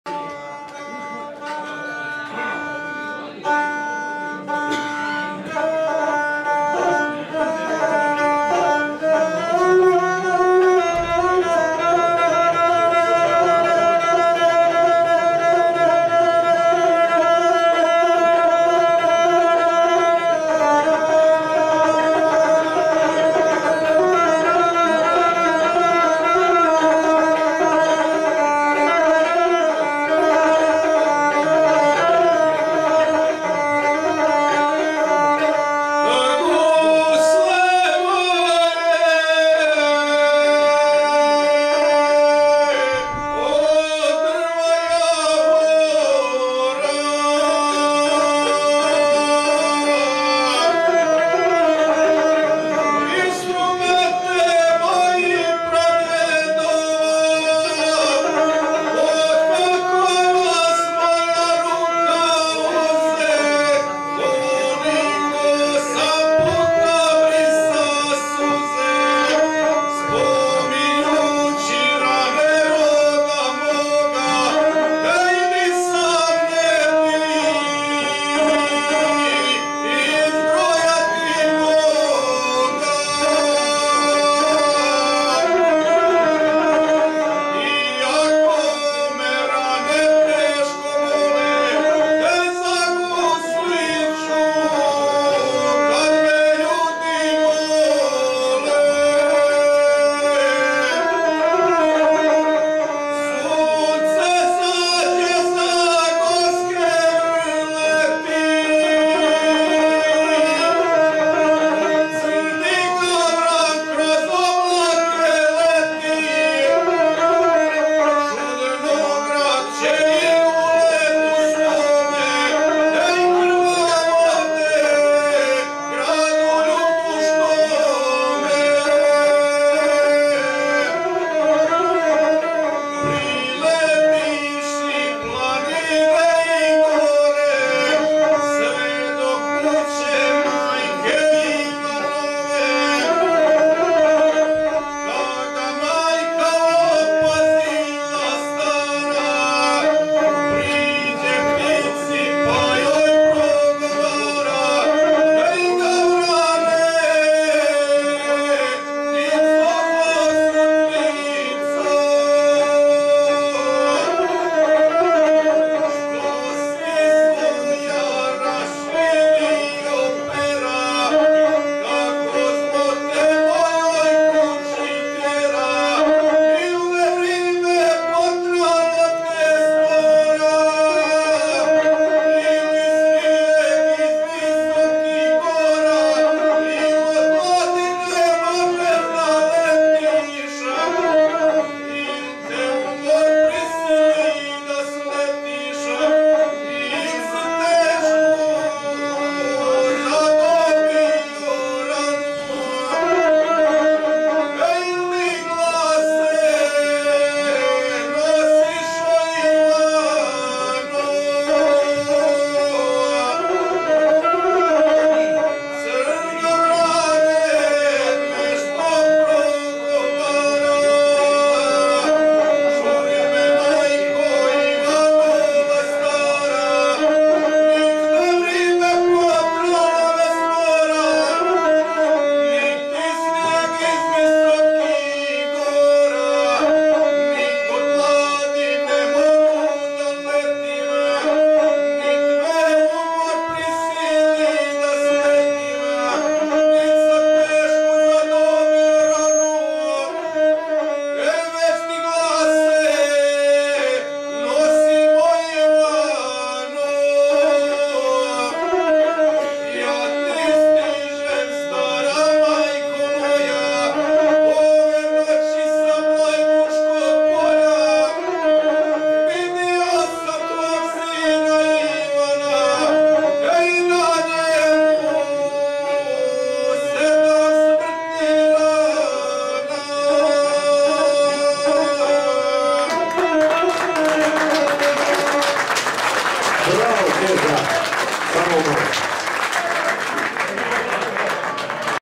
Hrvatski guslar-Željko Keža
14 večer guslara gangaša i diplara kod Žaera...